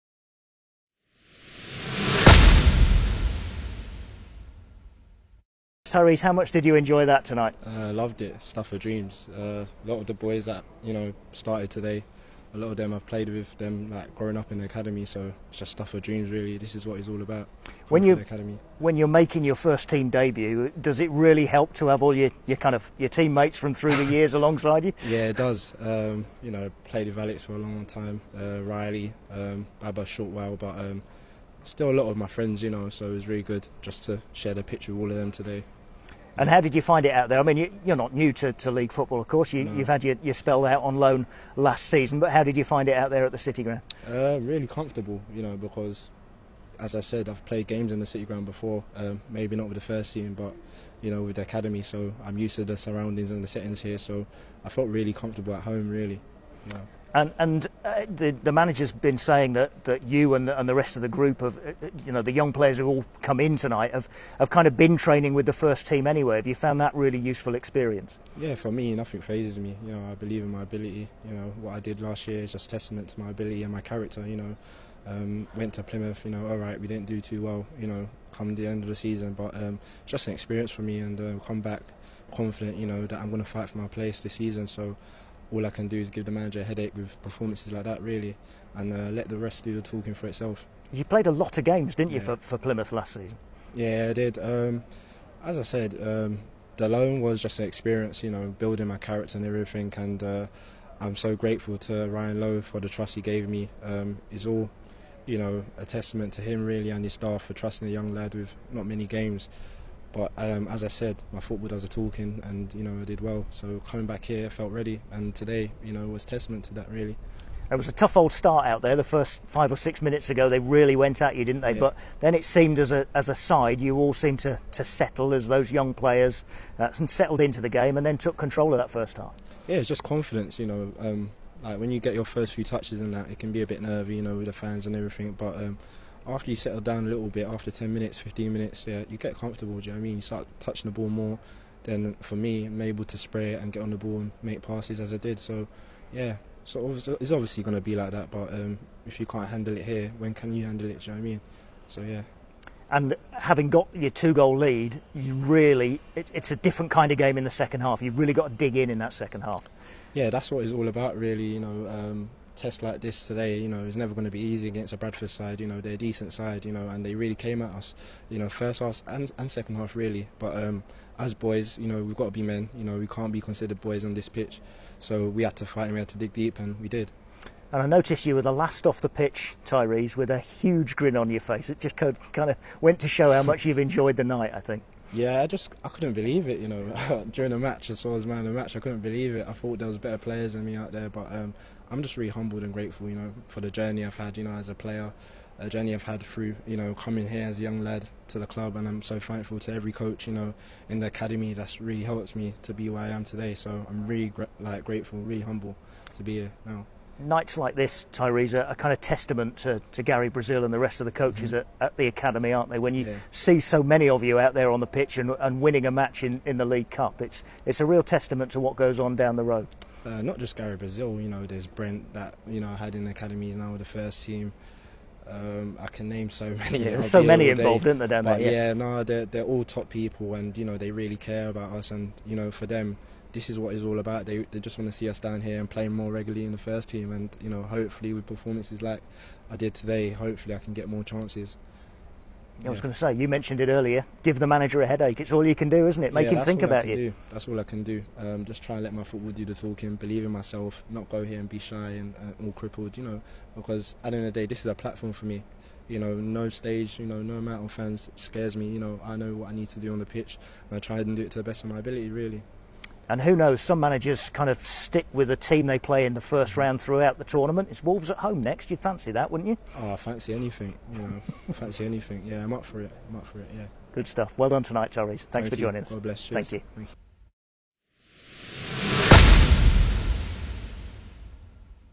Post-Bradford (H): Tyrese Fornah
Nottingham Forest midfielder Tyrese Fornah shares his views following the Carabao Cup win at home to Bradford City.